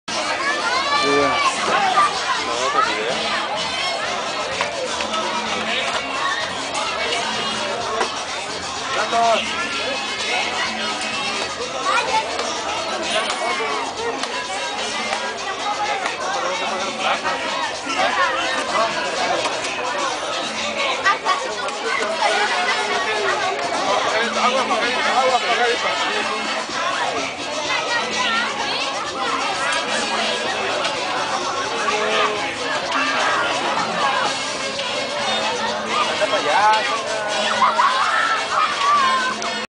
Fiestas Patronales En La Colonia Felipe Angeles
un pequeño video de el dia de la fiesta en la colonia felipe angeles.